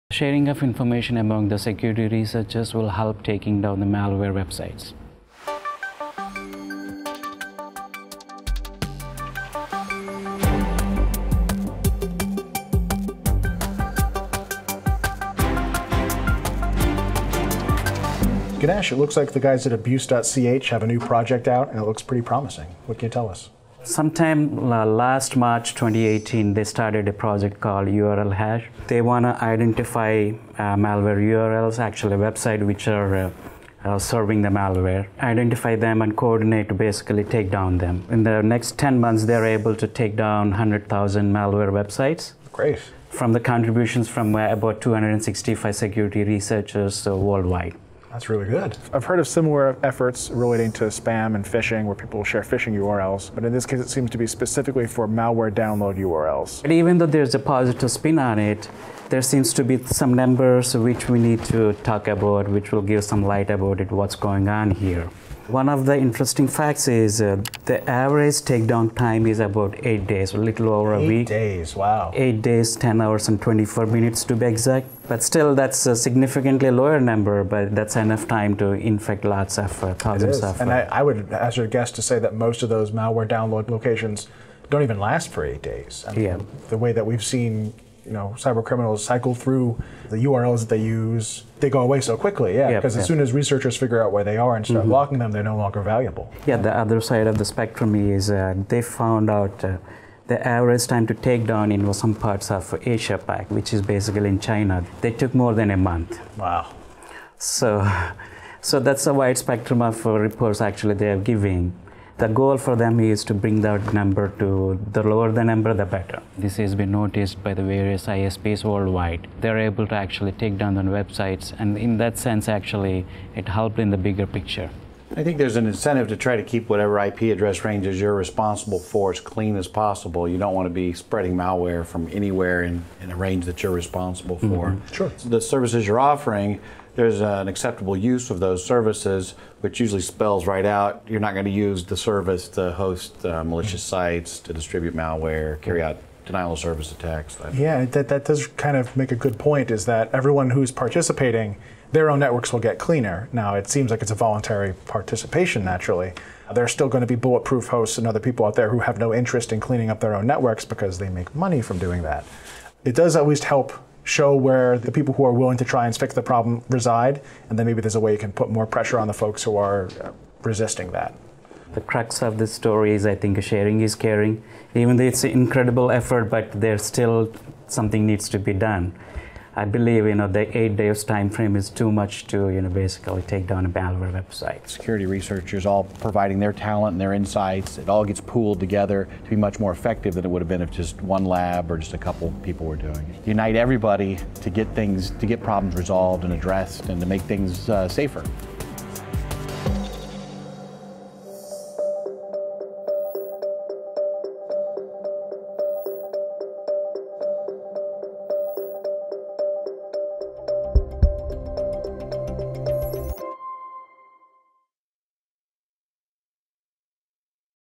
1/31/19 Security Researchers Take Down 100K Malware Sites
Originally recorded January 29, 2019

AT&T Data Security Analysts Matt Keyser, Ganesh Kasina and Michael Singer discuss the week's top cyber security news, and share news on the current trends of malware, spam, and internet anomalies observed on the AT&T Network.